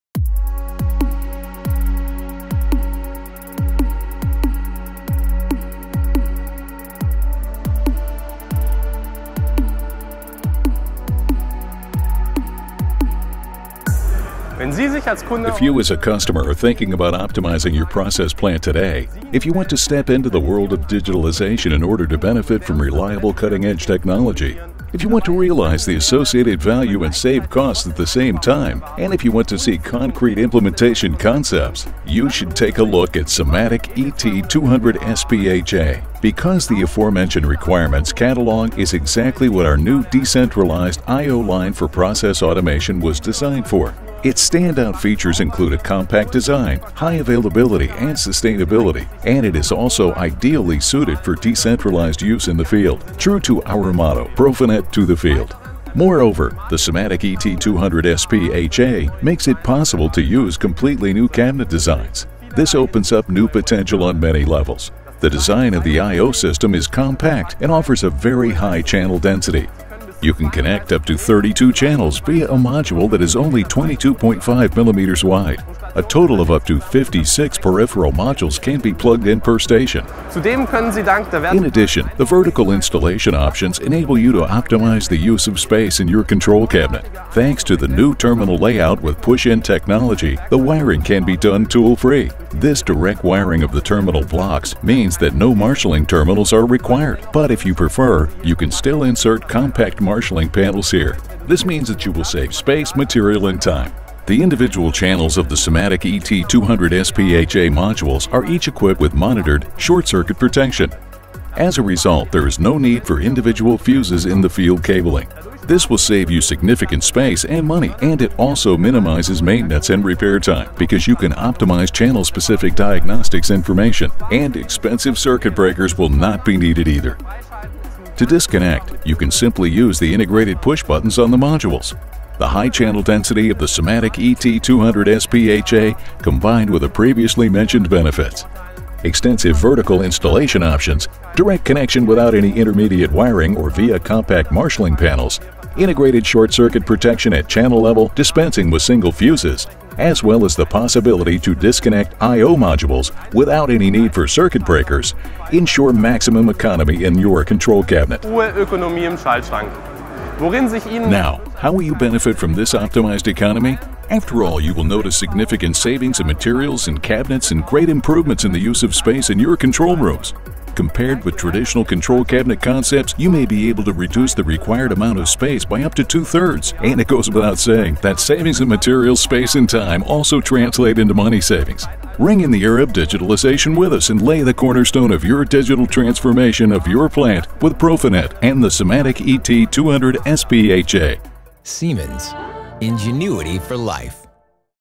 SIMATIC ET 200SP HA: new cabinet designs with the high performance I/O
SIMATIC ET 200SP HA is the high-performance I/O for the process industries. Thanks to compact design, high-availability and investment protection, the I/O system can not only be used directly in the field but enables completely new control cabinet designs. That's how you realize new potentials for the digital transformation of your plant!